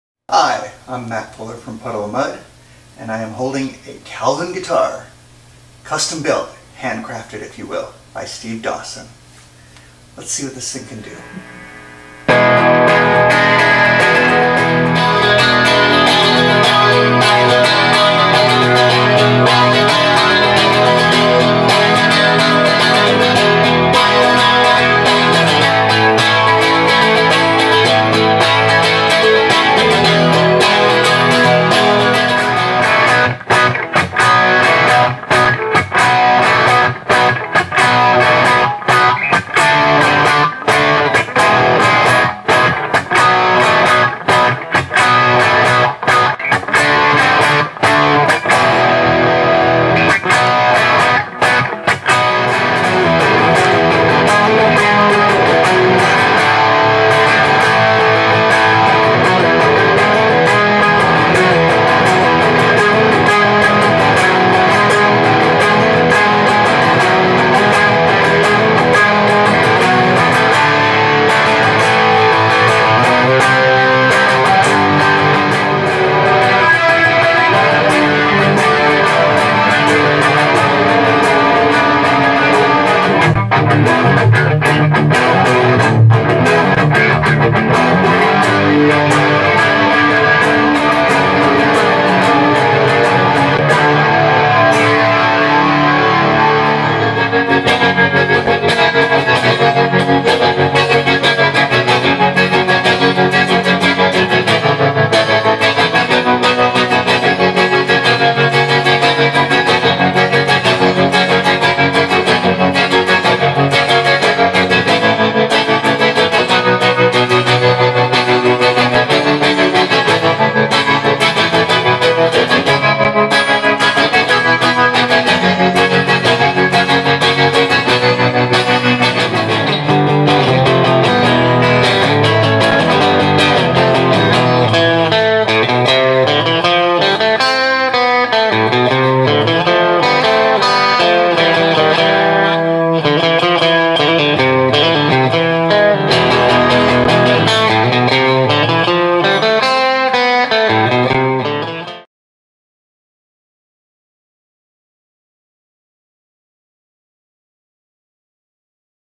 Matt Fuller/Puddle Of Mudd - on the CALVIN GUITAR! AVAILABLE in 2023!
CALVIN GUITARS - now AVAILABLE in 2023!

Created by Steve Dawson in Los Angeles, CALVIN GUITARS honor that traditional & classic design but are embellished with a bit of glam & modifications - all at a very affordable & fair price!

Featuring Matt Fuller from Puddle Of Mudd.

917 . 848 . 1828